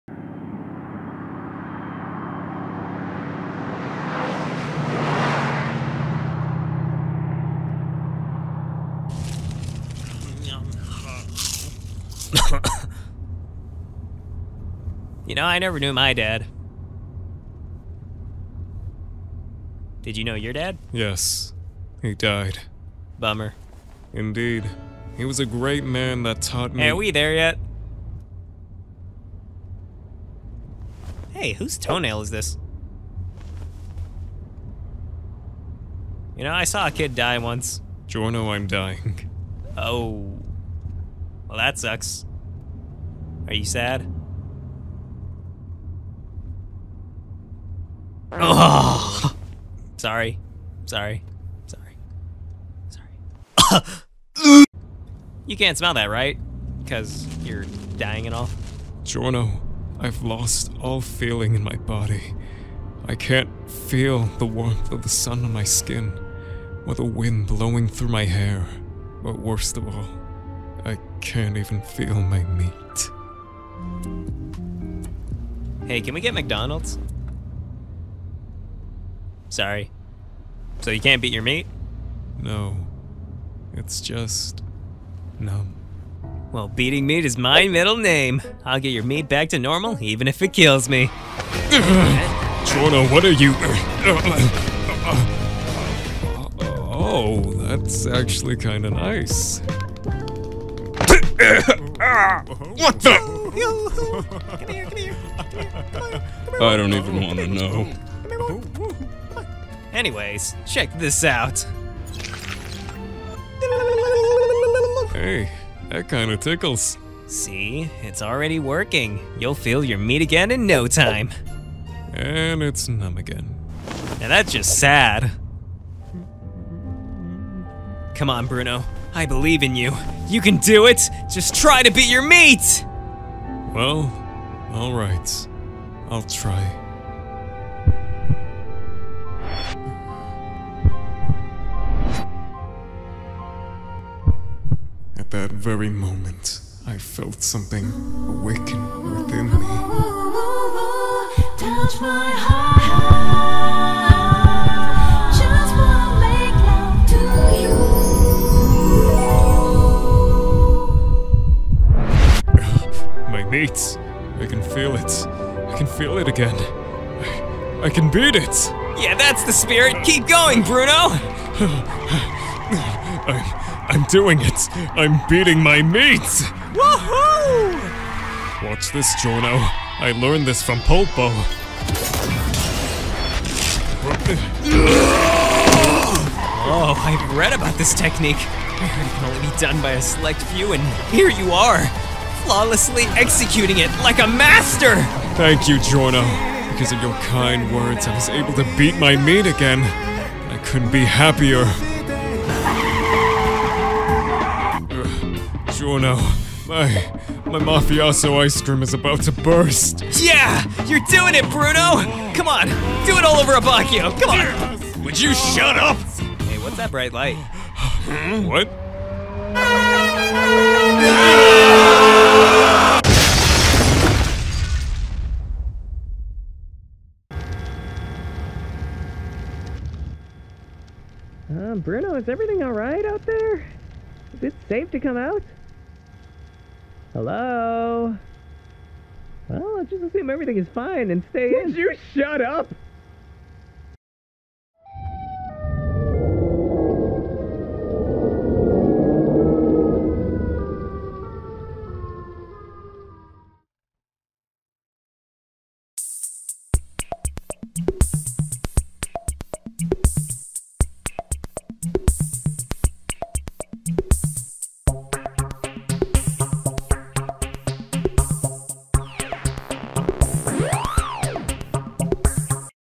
Bruno Beats His Meat
STICKYYY FINGERS!!!

My Favorite JoBro, Bruno! And The Longest Beat Meat And Animation On This Channel So Far!

Also Quick Note; Video Was Done Within 14 Days! 🐴

Voice Cast:

@ChrisCrossYT - Giorno Giovanna - Bruno Bucciarati - Narancia Ghirga

@CherryBoyYT  - Leone Abbacchio - Guido Mista